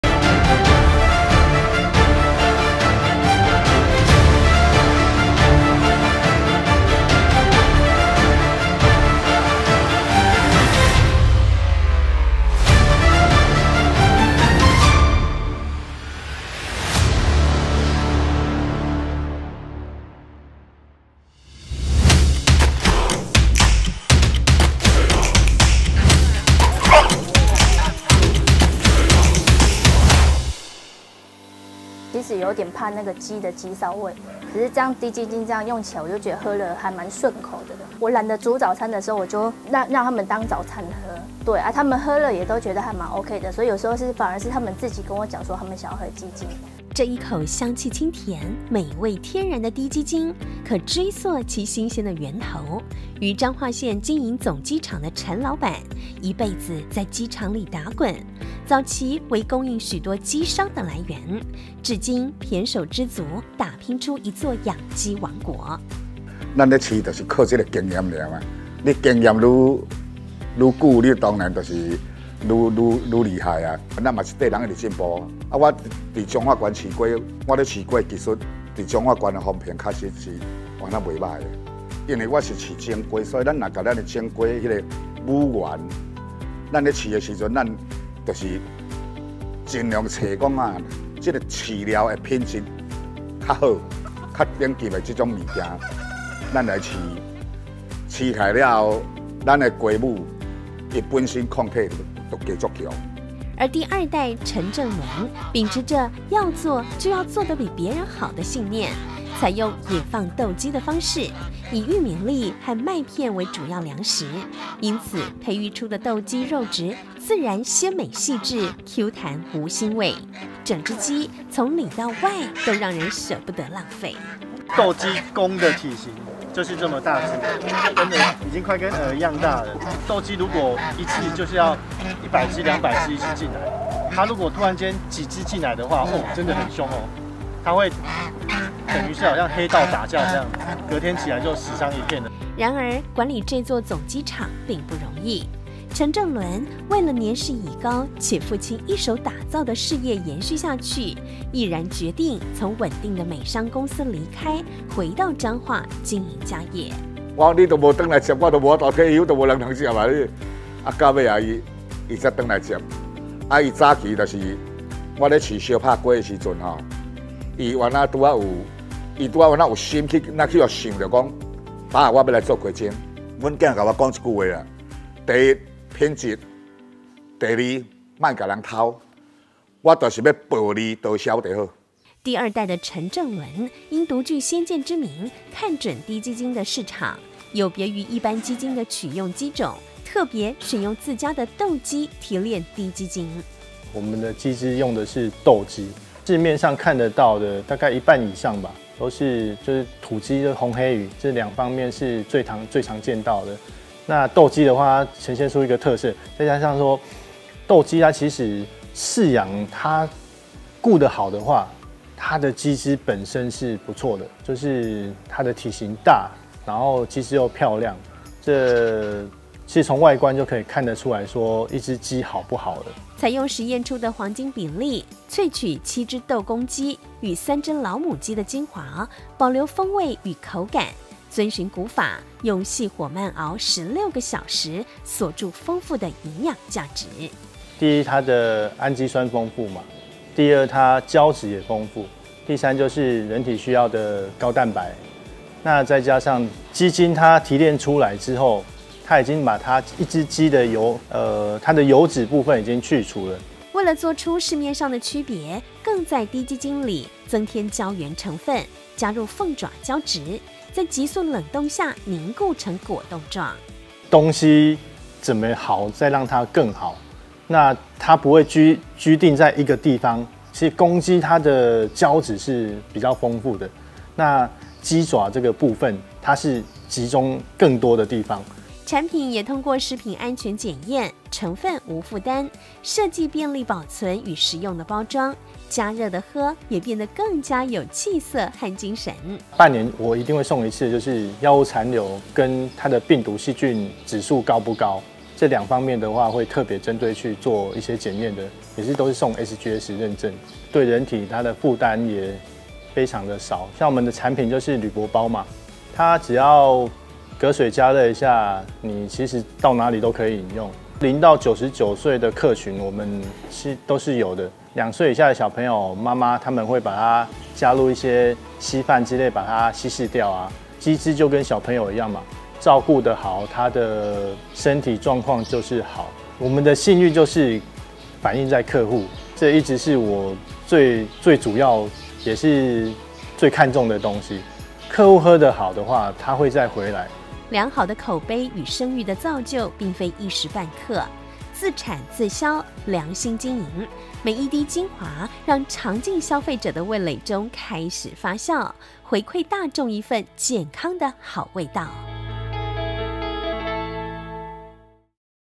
無距國度&凰膳滴雞精
這一口香氣清甜、美味天然的滴雞精可追溯其新鮮的源頭，於彰化縣經營總雞場的陳老闆，一輩子在雞場裡打滾，早期為供應許多雞商的來源，至今胼手胝足打拼出一座養雞王國。
而第二代陳正倫秉持著要做就要做得比別人好的信念，採用野放鬥雞的方式，以玉米粒和麥片為主要糧食，因此，培育出的鬥雞肉質自然鮮美細緻，Q彈無腥味，整隻雞從裡到外都讓人捨不得浪費。
遵循古法，用細火慢熬十六個小時，鎖住豐富的營養價值。